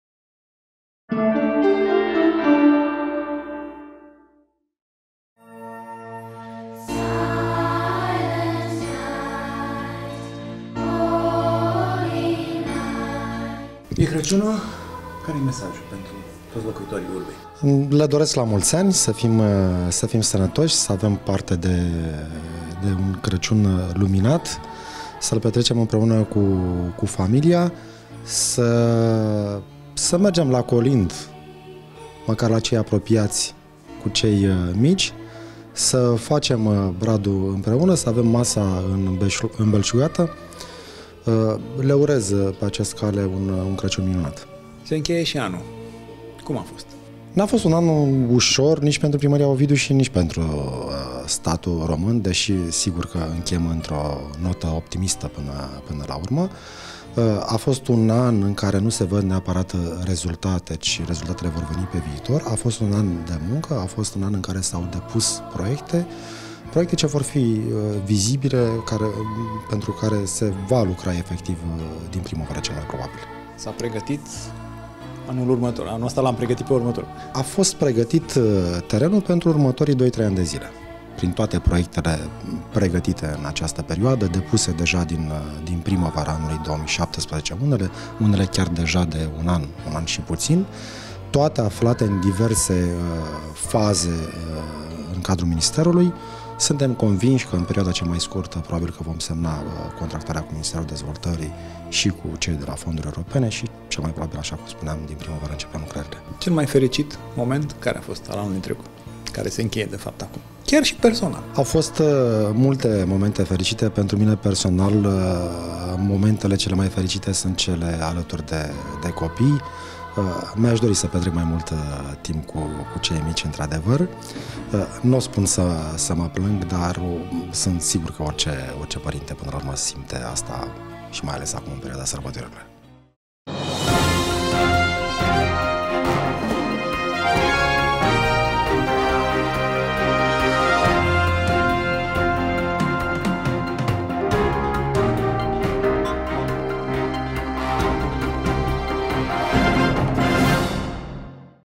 ctnews.ro | George Scupra, primarul din Ovidiu - mesaj de Sărbători
Primarul orașului Ovidiu, George Scupra, transmite un mesaj de Crăciun